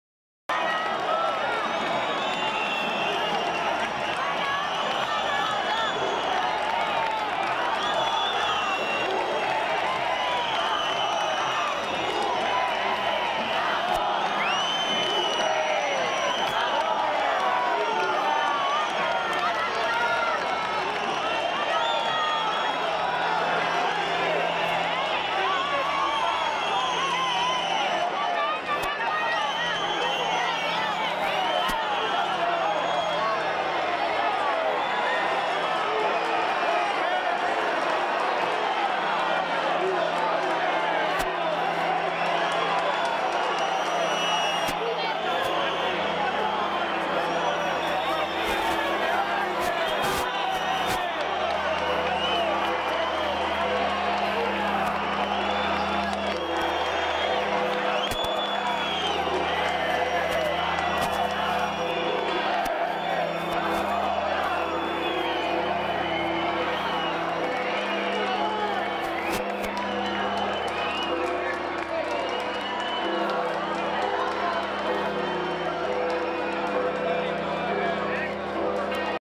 Miembros de la izquierda abertzale escupen y empujan al alcalde de Pamplona
Numerosos miembros de la izquierda abertzale han protagonizado una agresión contra el alcalde y los concejales de Pamplona durante la procesión de San Fermín, con zarandeos, insultos y empujones que han provocado la caída al suelo de una de las ediles y han dejado también tres policías heridos.

(Fuente: Navarra Televisión)